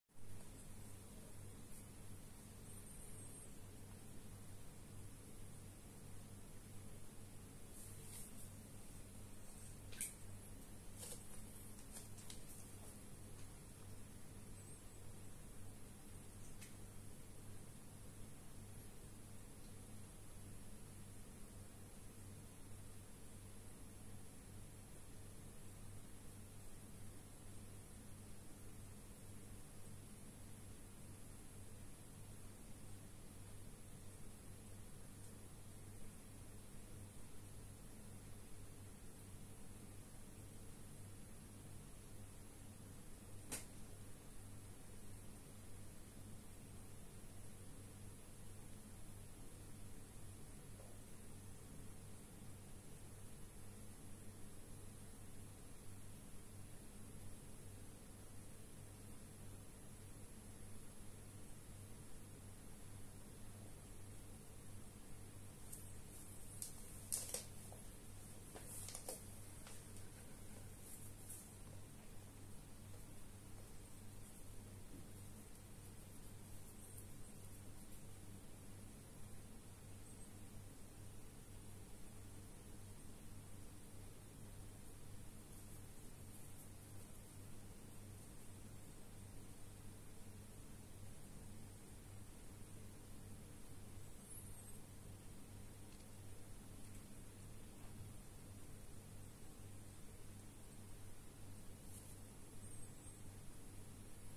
Кормление рыбок замороженным аулофорусом
Кормление рыбок замороженным аулофорусом. Хорошая тема когда червя слишком много. Хотя аулофоруса можно спокойно держать в живом виде, в небольшом количестве.